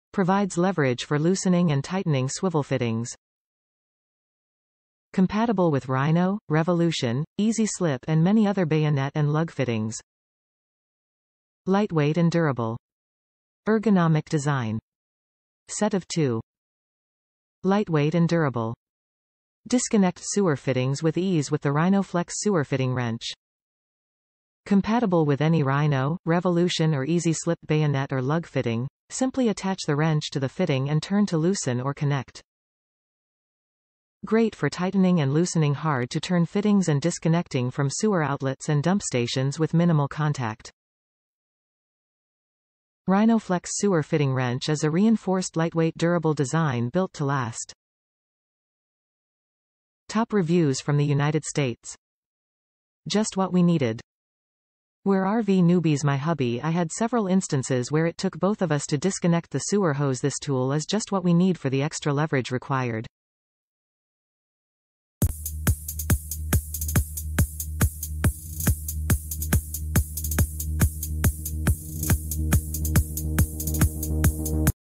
Camco Rhinoflex RV Sewer Fitting Wrench Set- Helps You Grip and Loosen or Tighten Swivel Fittings
Provides leverage for loosening and tightening swivel fittings.
Compatible with Rhino, Revolution, Easy Slip and many other bayonet and lug fittings
Lightweight and durable
Ergonomic design
Set of 2
Lightweight and durable
Disconnect sewer fittings with ease with the RhinoFLEX Sewer Fitting Wrench. Compatible with any Rhino, Revolution or Easy Slip bayonet or lug fitting, simply attach the wrench to the fitting and turn to loosen or connect. Great for tightening and loosening hard-to-turn fittings and disconnecting from sewer outlets and dump stations with minimal contact. RhinoFLEX Sewer Fitting Wrench is a reinforced lightweight durable design built to last.

Just what we needed
We’re rv newbies & my hubby & I had several instances where it took both of us to disconnect the sewer hose this tool is just what we need for the extra leverage required.